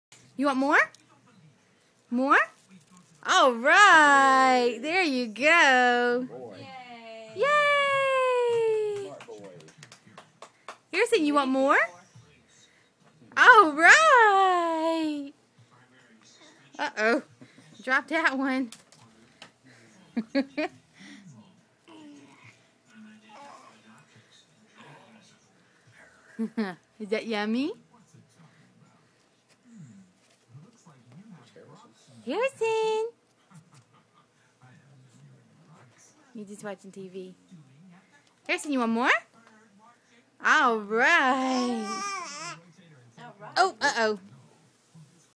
Harrison signs "more"
I learned how to let Mommy and Daddy know when I want more crackers.